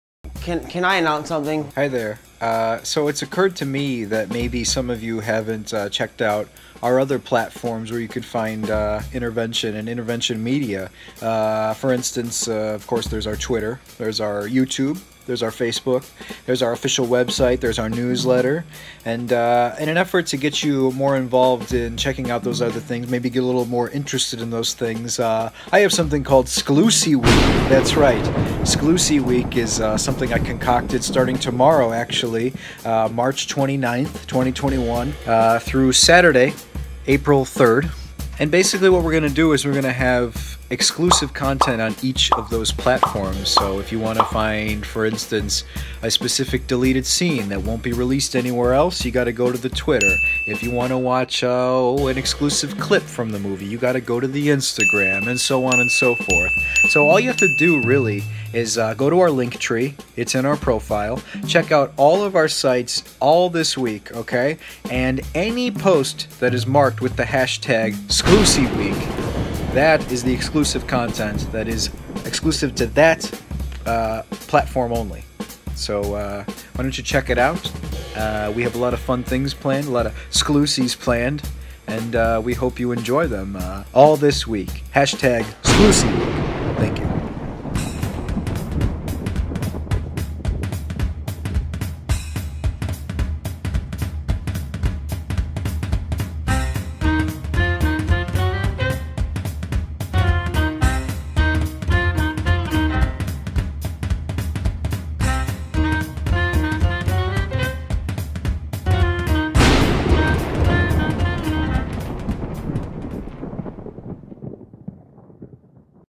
'Sclussy Week Kick-Off!
What is sclussyweek ??? Well, take a look at the latest video from director Dan Eden for the rundown. It's basically a project that lasts all throughout this week (3/28-4/3) in an effort to get you engaged in our other platforms. You'll have to visit, say, our YouTube channel for exclusive--sorry--'sclussy content, like a behind the scenes featurette. Or perhaps if you want access to the latest deleted scene, you'll need to be brave and venture out to our Twitter page. I know, it's scary... but it's worth it! It's sclussyweek! and we have some 'sclussy goodies lined up for you! How do you know it's a genuine 'slussy? It'll be the only post (after this one) to have the sclussyweek hashtag on it. How do you know where to go? Our trusty LinkTree, which is down below! Any other questions? Okay then! Let's have a great sclussyweek everyone! Starting tomorrow, we will release one 'sclussy on one of the platforms... Hmm... which one will it be?